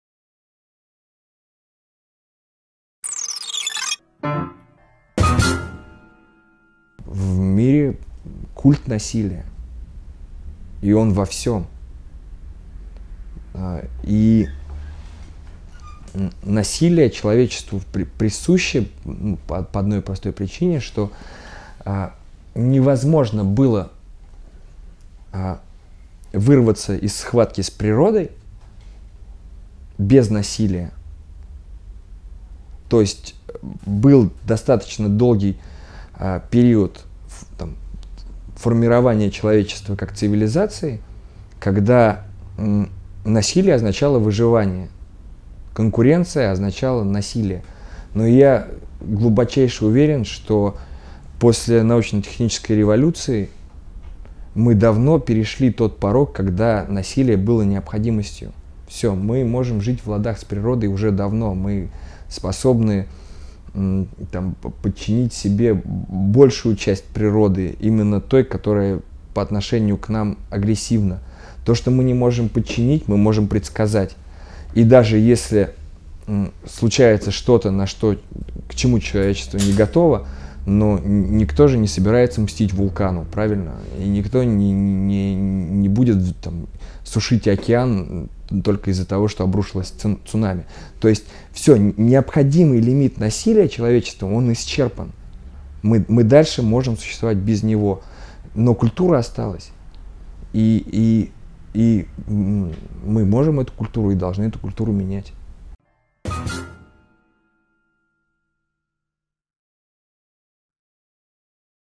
О культе насилия
Рустем Булатов - о том, почему необходимый лимит насилия в обществе исчерпан.

Новый герой МЫSLей – Рустем Булатов (Тэм). Музыкант, вокалист и автор текстов группы Lumen, основанной в 1998 году в Уфе. Учился на юрфаке, но бросил незадолго до окончания, так как профессия юриста показалась ему «не очень чистой» в моральном и духовном плане. Женат, воспитывает двух сыновей.